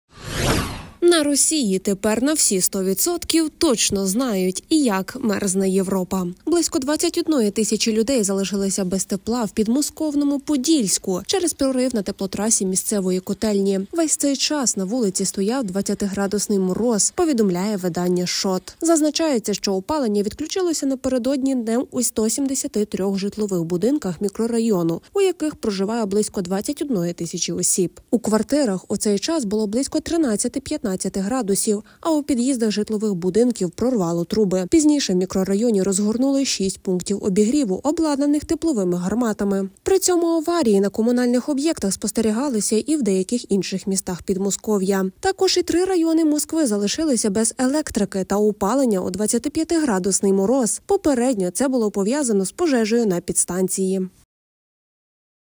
❄️На росії в -20° НА СМЕРТЬ ЗАМЕРЗАЮТЬ 20 тисяч росіян
На росії тепер на всі 100% точно знають, як мерзне Європа. Близько 21 тисячі людей залишилося без тепла в підмосковному Подільську через прорив на теплотрасі місцевої котельні. Весь цей час на вулиці стояв 20-градусний мороз, повідомляє видання SHOT.